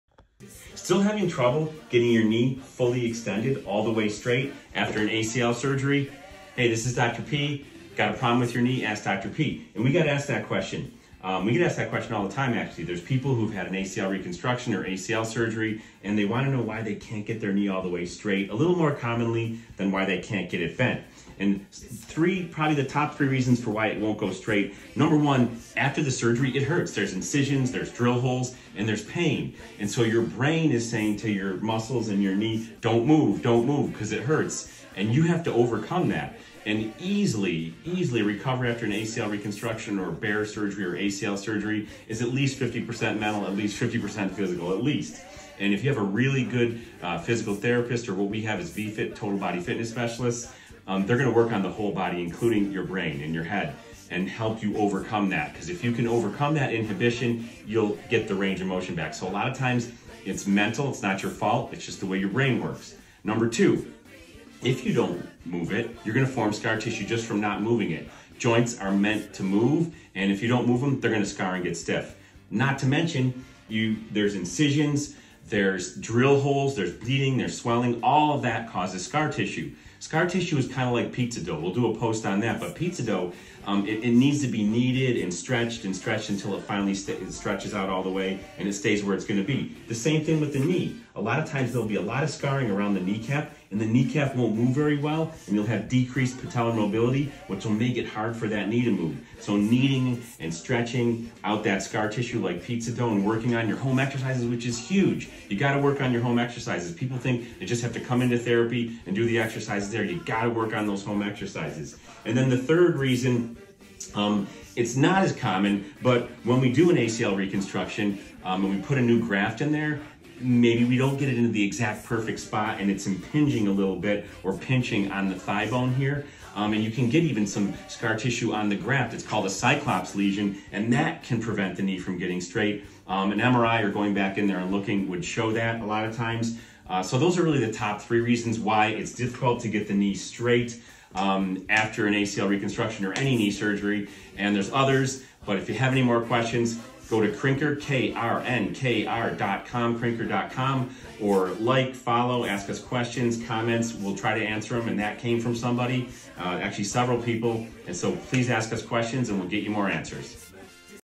Why is my knee stiff after ACL surgery?
WHY WON’T MY KNEE GO STRAIGHT AFTER MY ACL SURGERY?

Still missing full extension after ACL surgery?

Here are the Top 3 Reasons why this can happen, even 6 months out that we just got asked about by a follower.

1. Pain inhibition in the brain (AMI):
Your brain subconsciously shuts down full muscle activation to think it’s protecting the knee. Overcoming this takes not just physical work — but mental retraining too.

2. Scar tissue formation (arthrofibrosis):
Scar tissue can act like sticky/tough “pizza dough,” blocking extension and range of motion if not remodeled over time with manual therapy, movement, and stretching.

3. Graft impingement (cyclops lesion):
Rarely, a small ball of scar tissue forms at the front of the knee, physically blocking extension. Can only be confirmed by MRI and/or arthroscopic reevaluation.

Bottom line:
Regaining full extension after ACL surgery isn’t just about stretching harder — it’s about retraining your brain, remodeling scar tissue, and ruling out mechanical blocks.

Got a problem with your knee? Ask Dr. P!